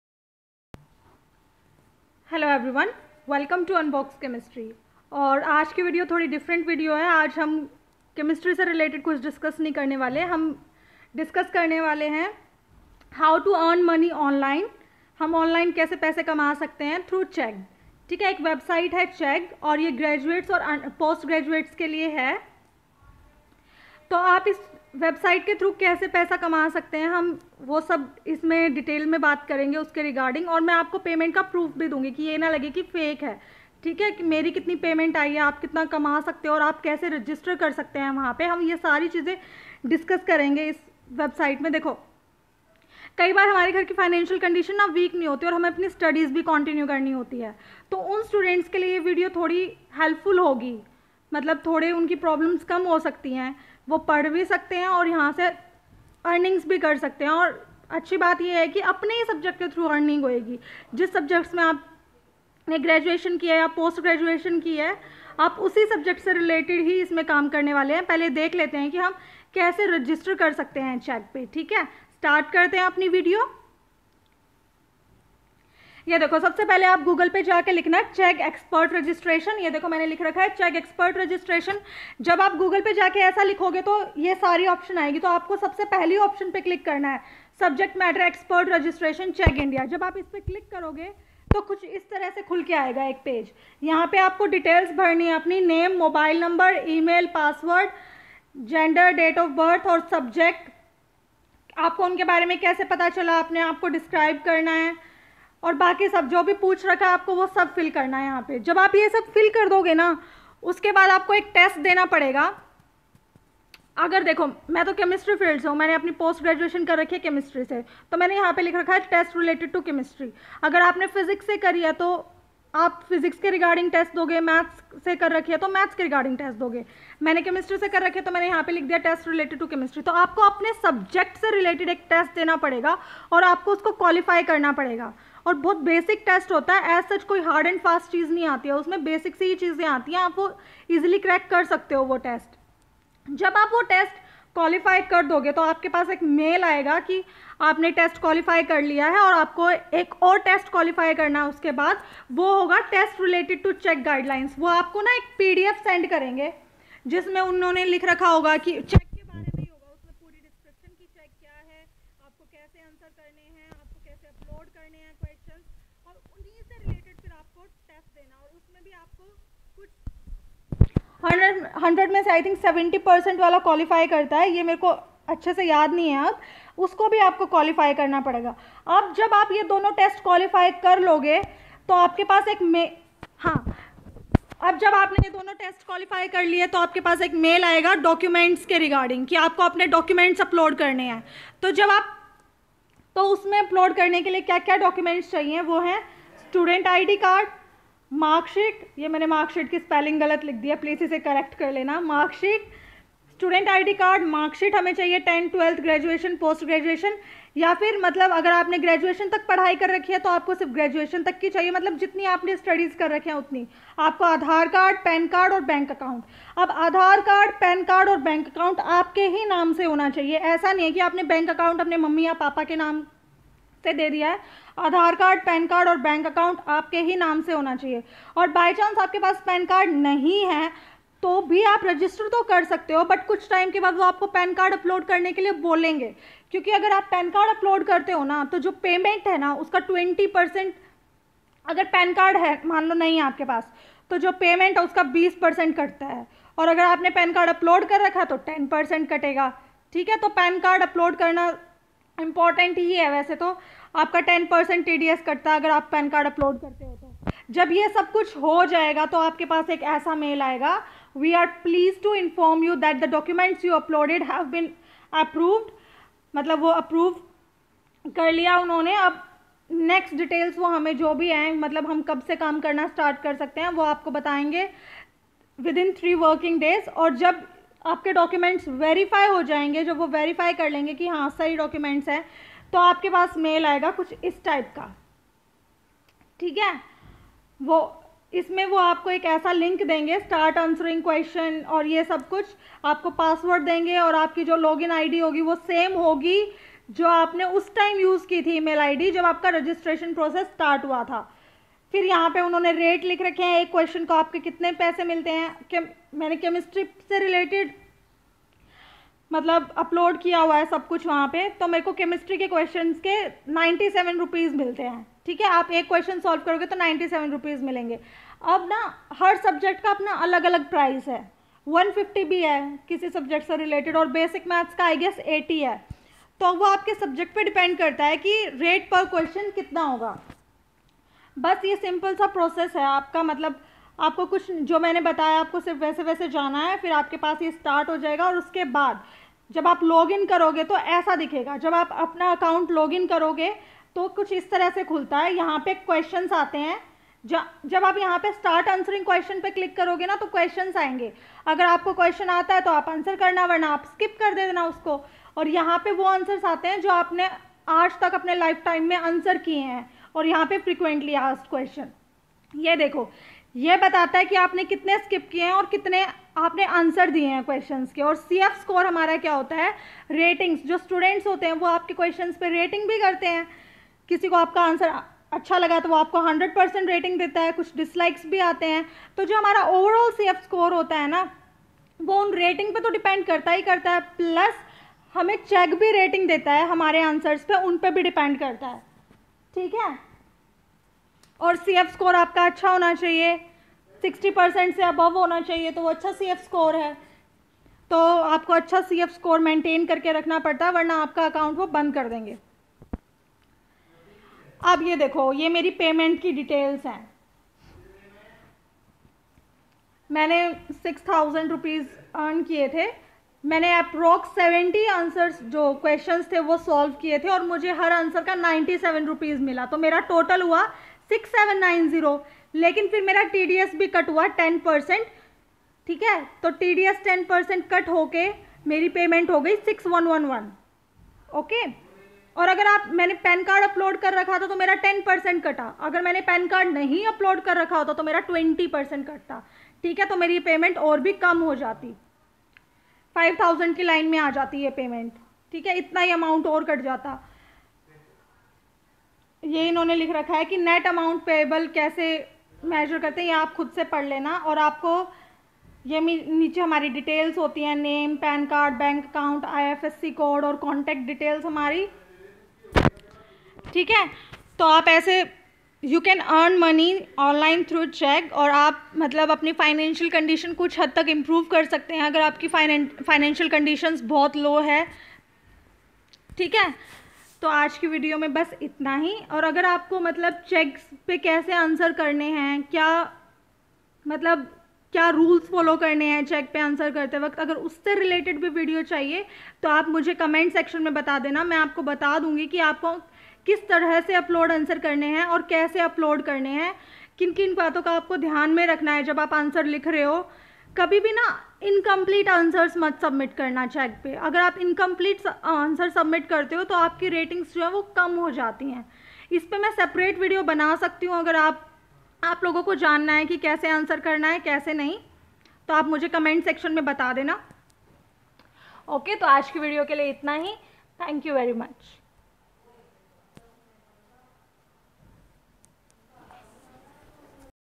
Chegg |Earn Money Through Chegg!!
It's an online platform for those students who wants to earn money as well as study.
If u have any query related to chegg than you may write it down in comment section i will try to answer all your queries.